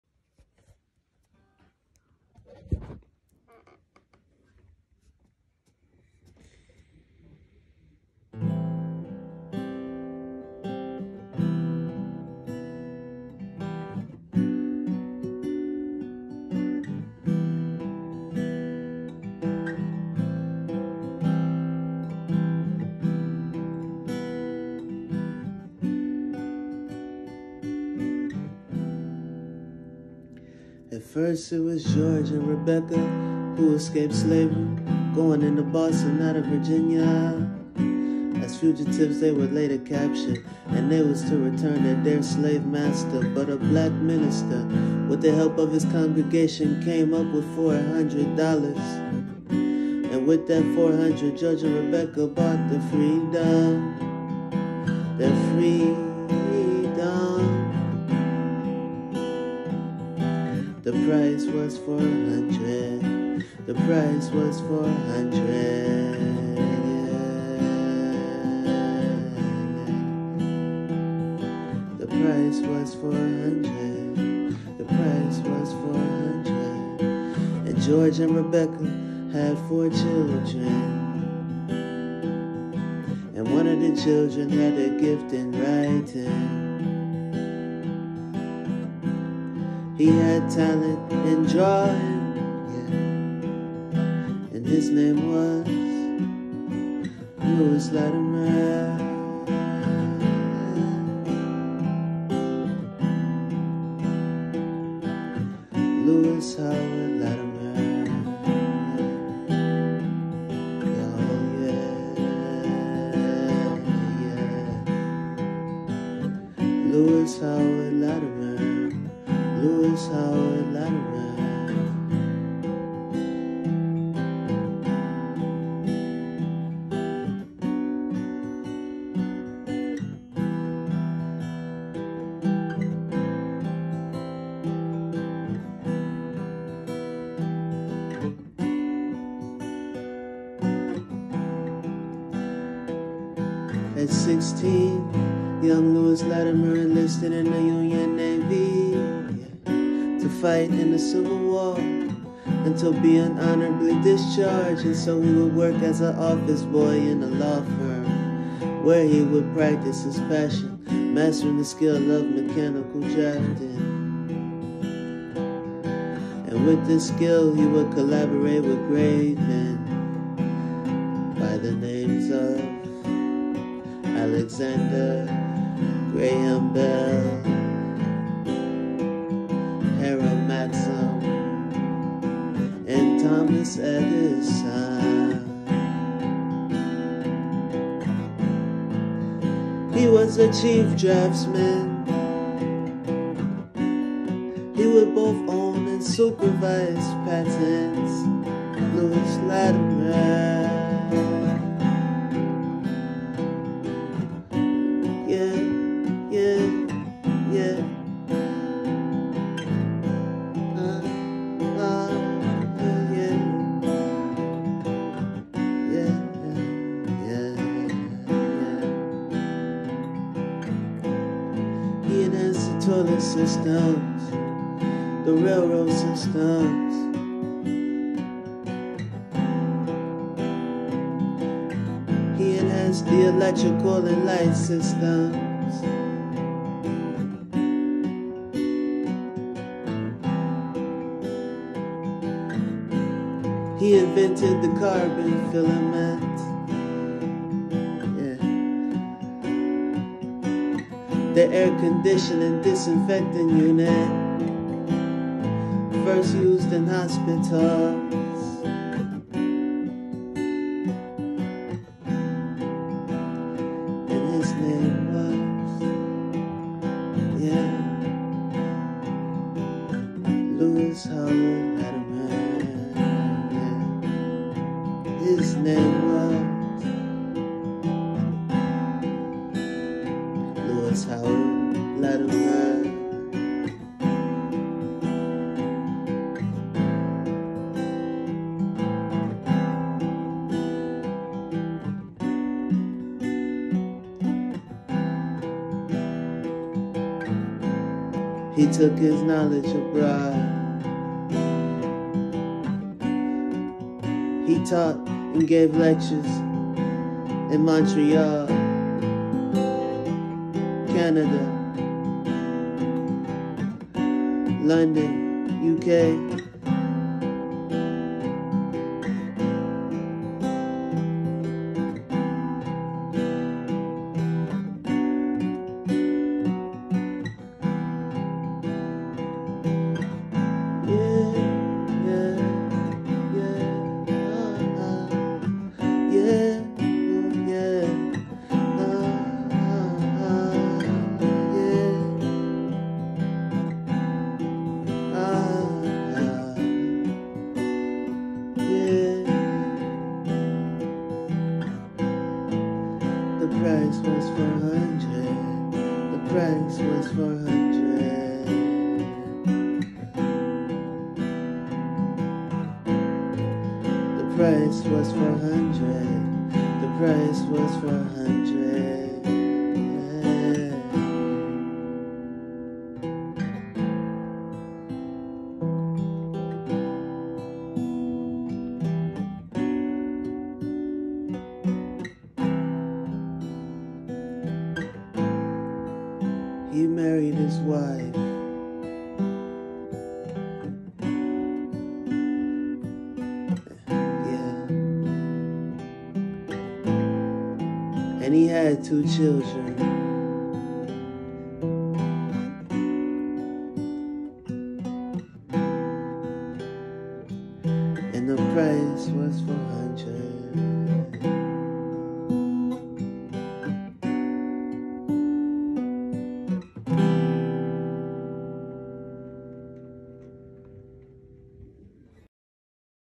Lewis Latimer Tribute
Here is my tribute to Lewis Howard Latimer; a great inventor, draftsman, patriot and contributor to the United States of America.

Video edited by Jalil Muhammad